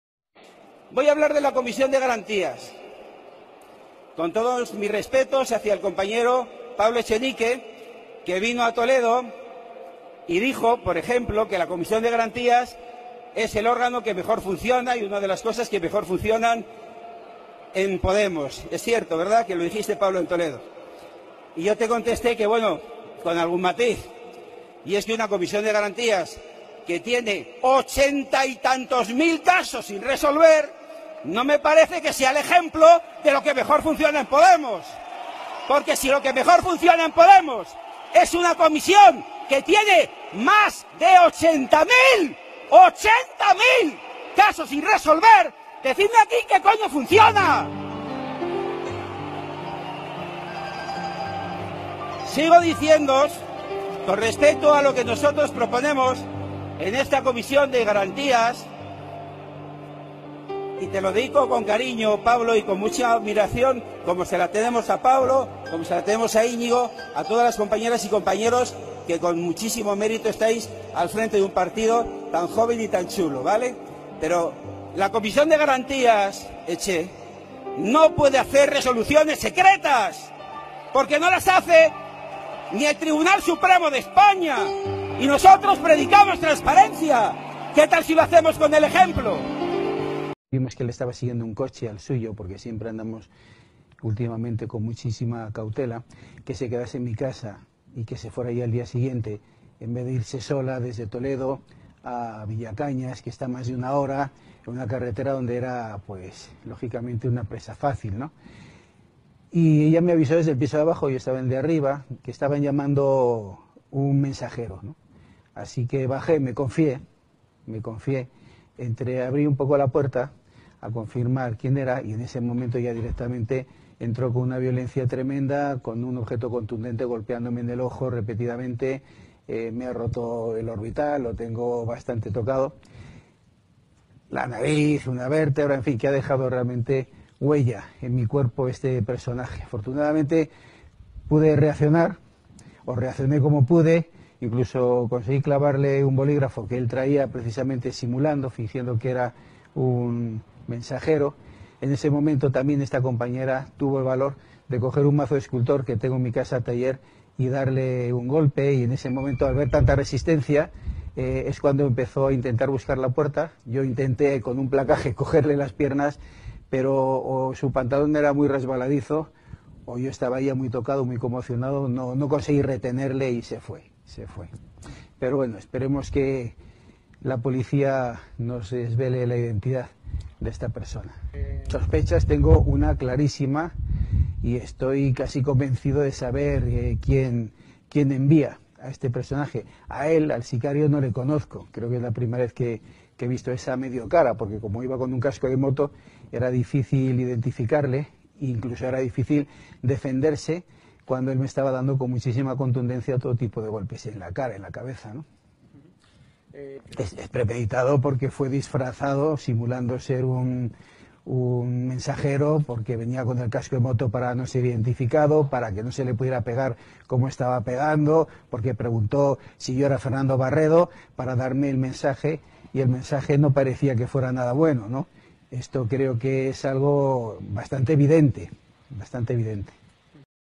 Así trata Podemos a sus críticos
Brutal paliza por criticar la transparencia del partido